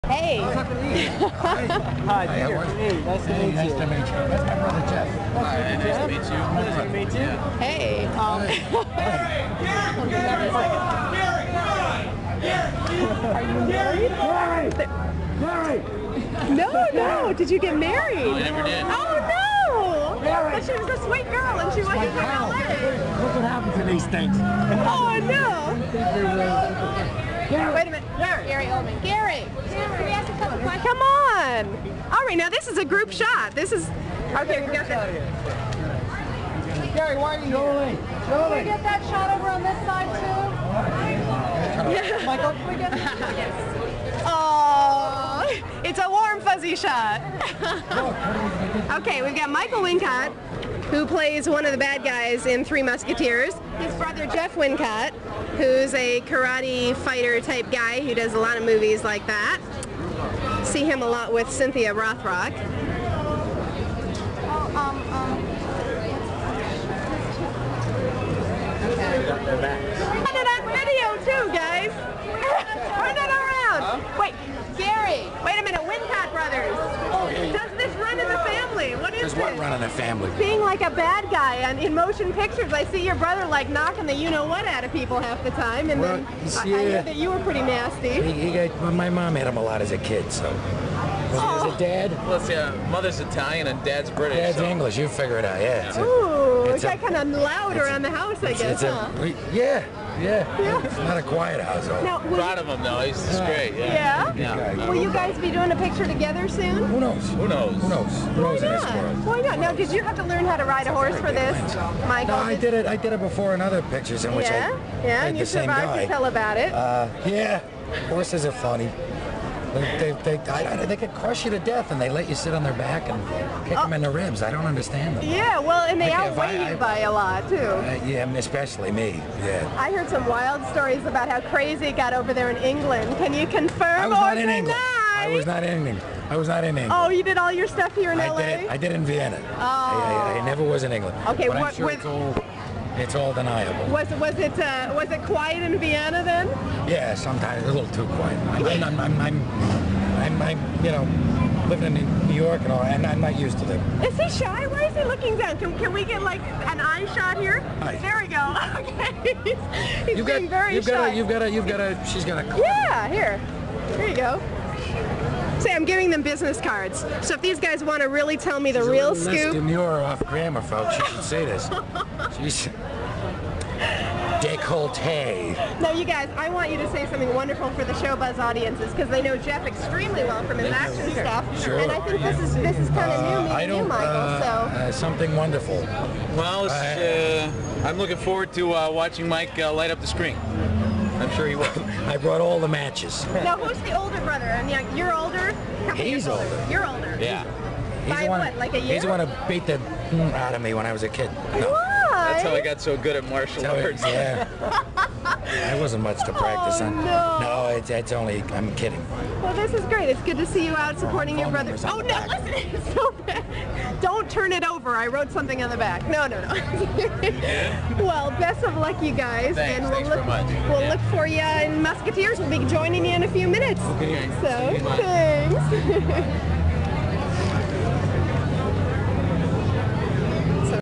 Michael Wincott and Gary Oldman meet at the premiere of 'The Three Musketeers' (1993)
Michael Wincott with his brother Jeff and Gary Oldman